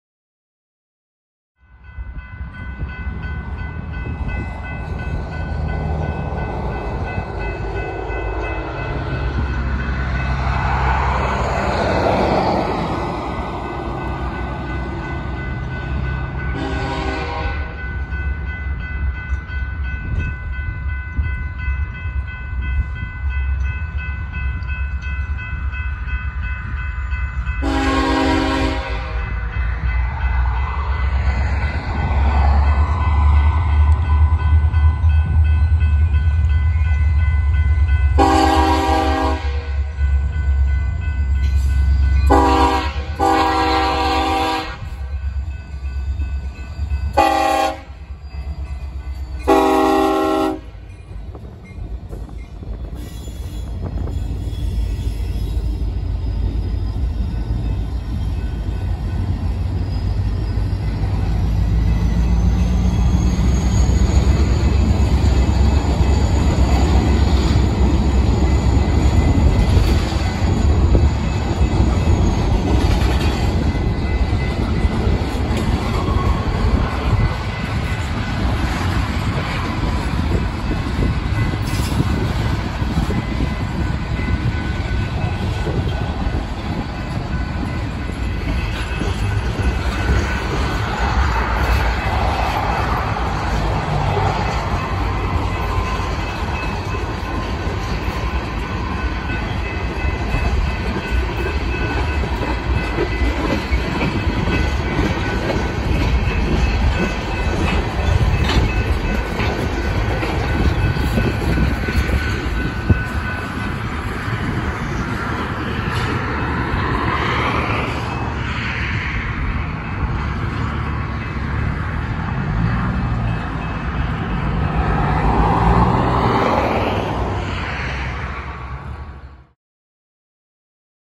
UP train LBJ77 arrives at Chico, TX (April 28, 2023)
UP #4847 has arrived at Chico with the LBJ77. Trailing behind the SD70M leader was UP #1881 (SD40N). The railcars would be dropped off at the siding and the power would be tied down further up the line.